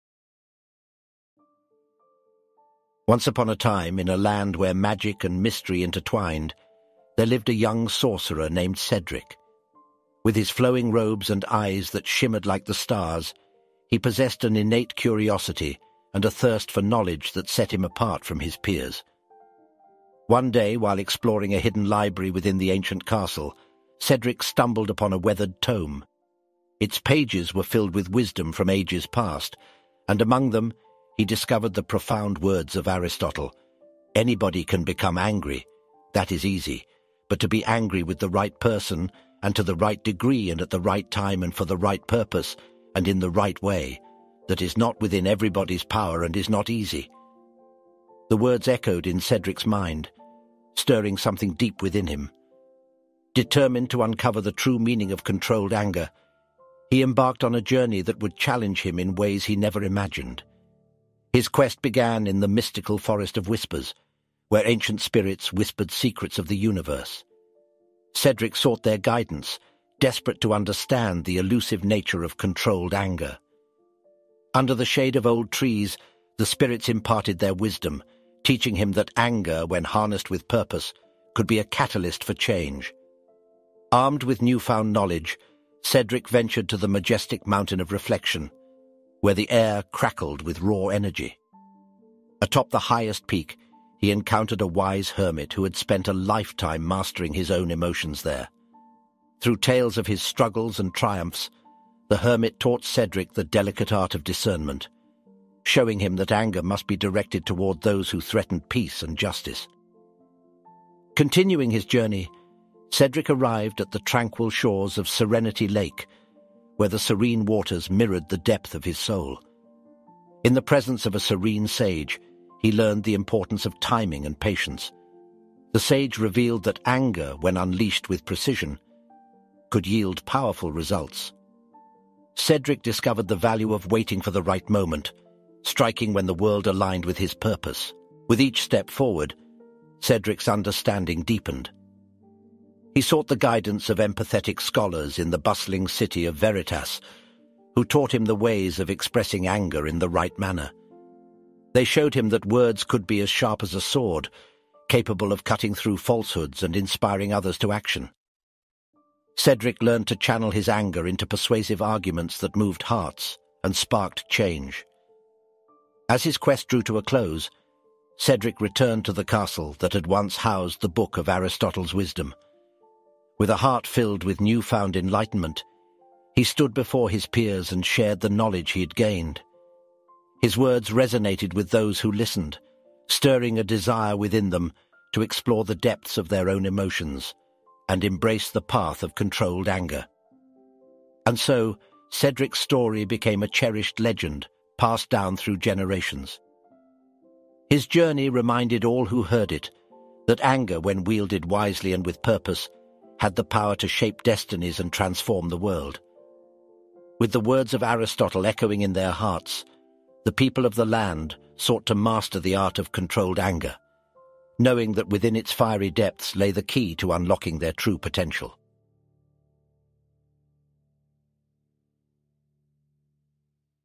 Unleashing the Flames Within | Cedric's Quest for Controlled Anger |Colourofstory | #002
In a world where magic and mystery intertwine, "Unleashing the Flames Within Cedric's Quest for Controlled Anger" takes readers on an enchanting journey. Join Cedric, a young sorcerer driven by curiosity and a desire to understand the depths of his own emotions, as he embarks on a transformative quest.

Guided by the profound words of Aristotle, Cedric delves into the realms of ancient knowledge, encountering mystical beings and traversing magical landscapes. From the ethereal Forest of Whispers to the serene shores of Serenity Lake, Cedric seeks wisdom from wise spirits, hermits, and scholars.

As Cedric's understanding deepens, he learns the intricacies of controlled anger. Through trials and challenges, he discovers that anger can be a powerful force for change when directed towards the right person and purpose, at the right degree and time, and in the right way. Cedric's journey is one of self-discovery, as he learns to harness his fiery emotions and shape them into a catalyst for transformation.

"Unleashing the Flames Within: Cedric's Quest for Controlled Anger" weaves together elements of fantasy, self-reflection, and personal growth. It explores the intricate dance between passion and restraint, reminding readers of the importance of mindfulness and intention in navigating their own emotions.

This captivating tale captivates readers with its vivid descriptions of enchanting landscapes, wise mentors, and the vibrant inner world of Cedric. Through his experiences, readers are invited to reflect on their own relationship with anger and the potential for growth and positive change that lies within.